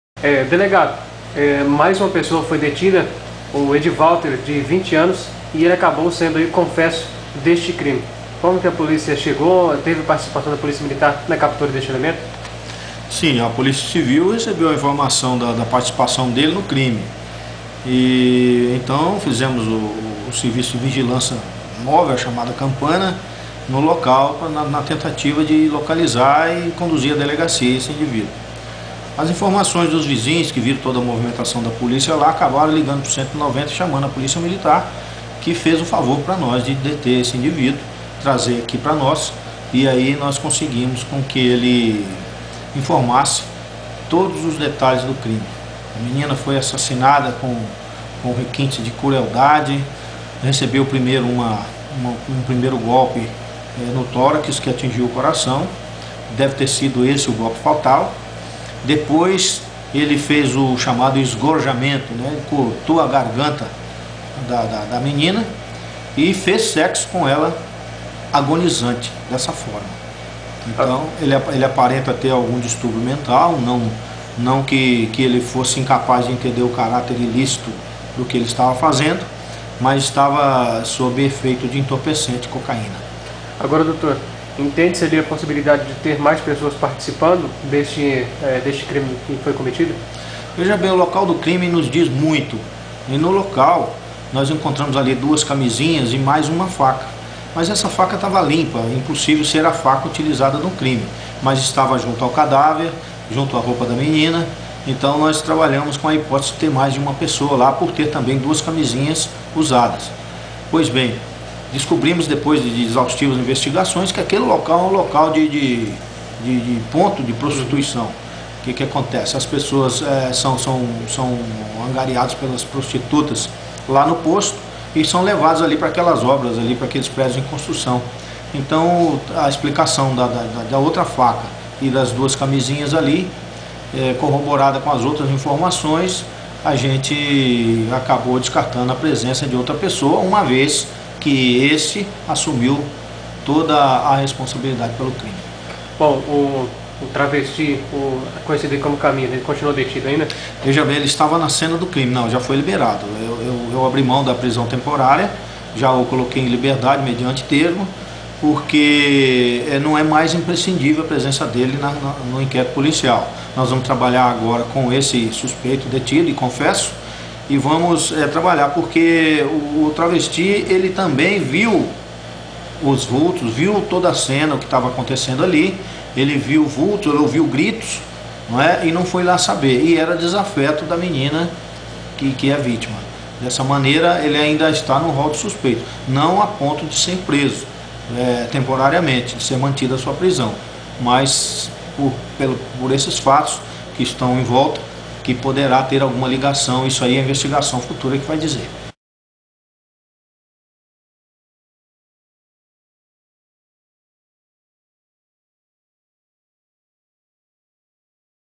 Assassino de Viviane confessa ao delegado que manteve relações sexuais com ela agonizando
Delegado Joáz Gonçalves disse para a Rádio Difusora Juara que Edivalter Alves Candido, 20, assassino confesso da jovem Viviane Pereira, não estava sozinho na sena do crime e que agiu com requentes de crueldade.

Dr. Joáz entende que o fato de serem encontrados dois preservativos usados no local do crime, aumenta a suspeita de que havia mais uma pessoa junto com o assassino.